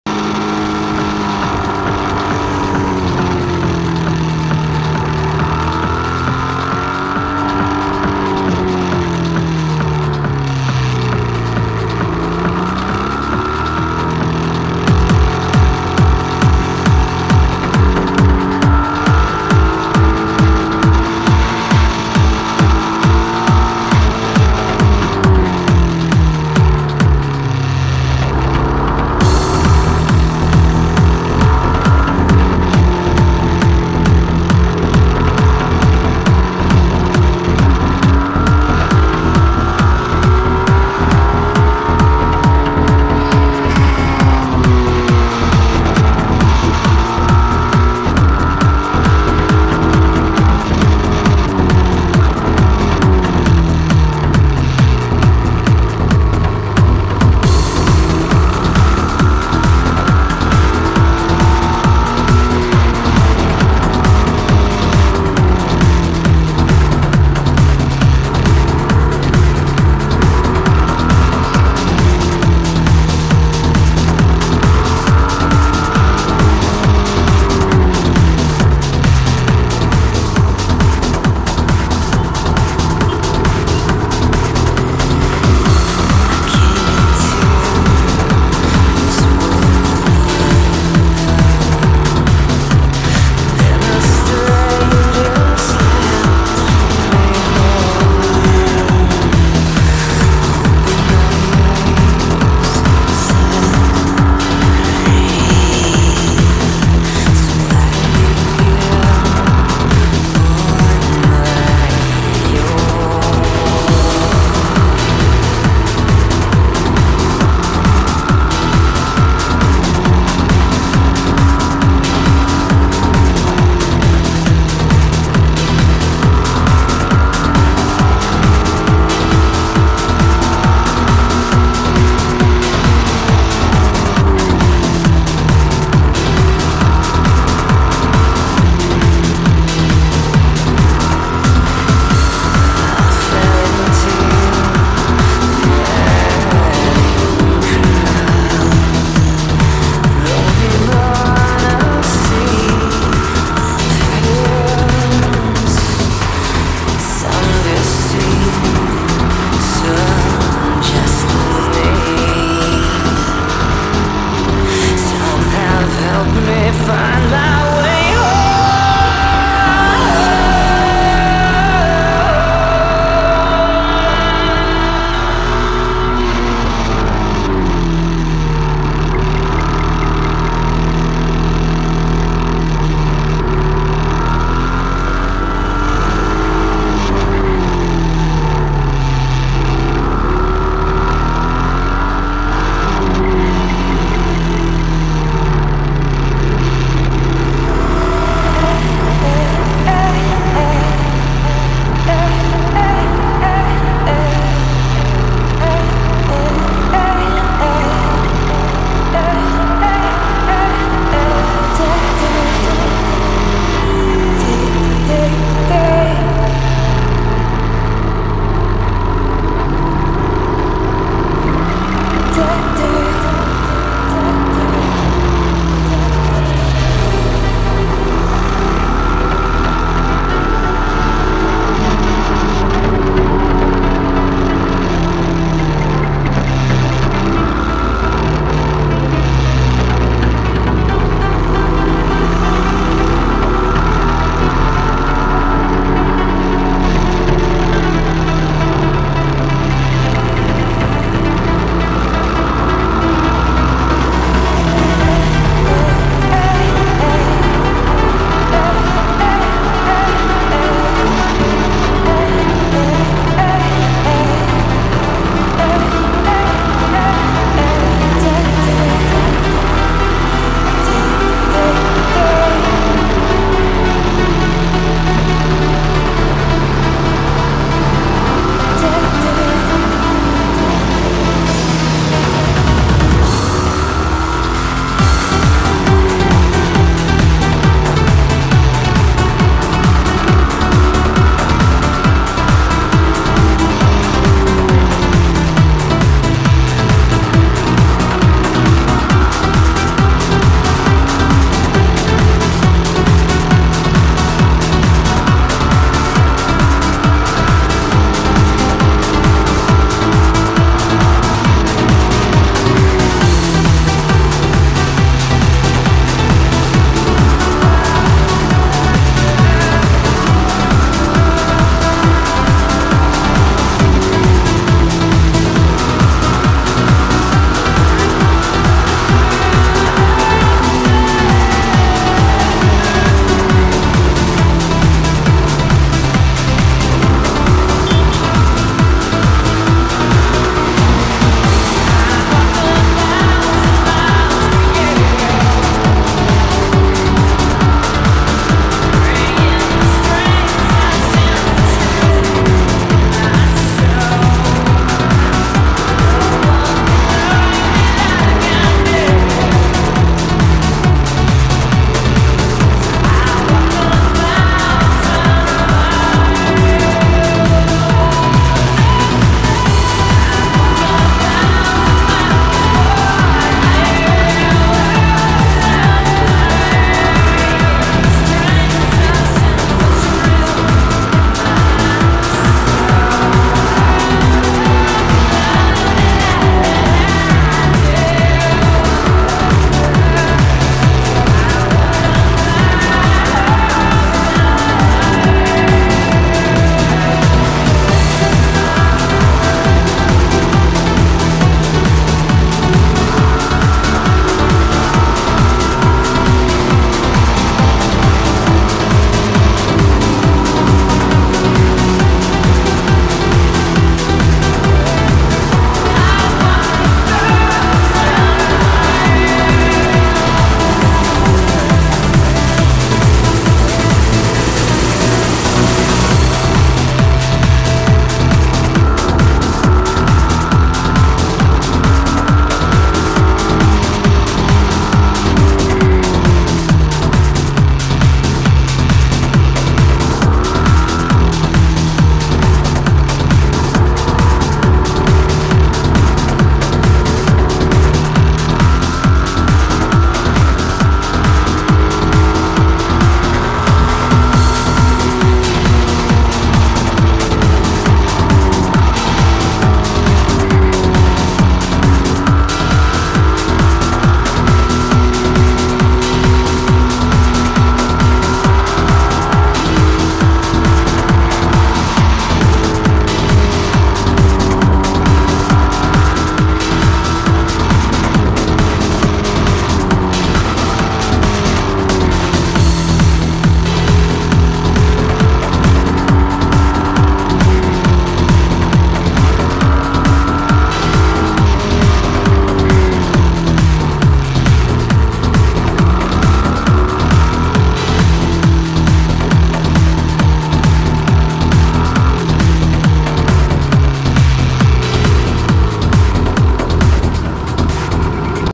Ducati Monster 1200S & Ducati Diavel - Yedigöller yolunda gazlama
2 Ekim 2016'da yaptığımız Bolu gezimizden. Ducatilerin virajlarda koptuğu anlar.. Monster 1200S'İ kullanan arkadaşım Yedigöller yolunun eksperi olmuş durumda... :)